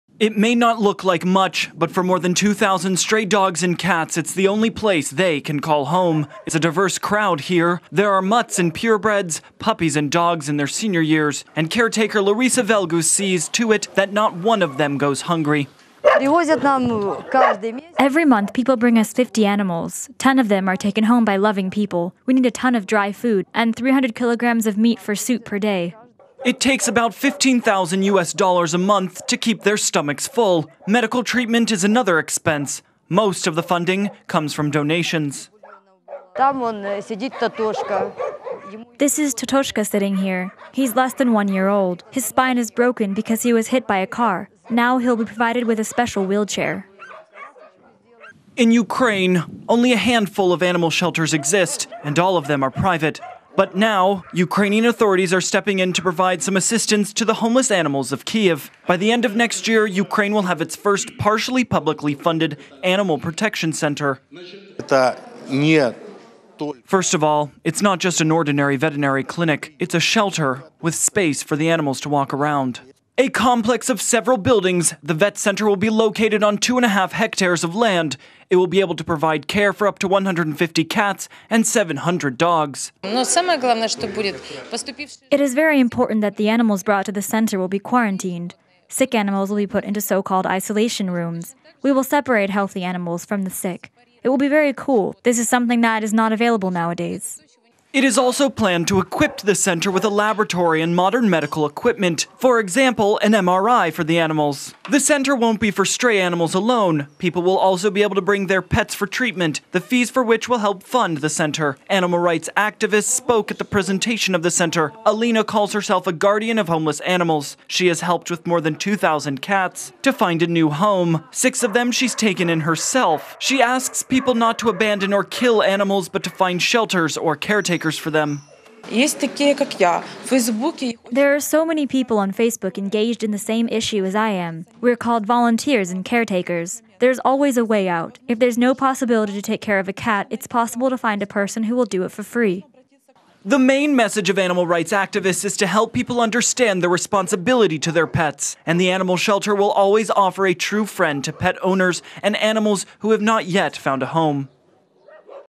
Every Dog Has Its Day: First Animal Protection Shelter to be Built in Kyiv
In Ukraine, only a few animal shelters exist. And all of them are private. But now, Ukrainian authorities are stepping in to provide some assistance to the homeless animals of Kyiv. By the end of next year, Ukraine will have its first partially publically funded Animal Protection Center.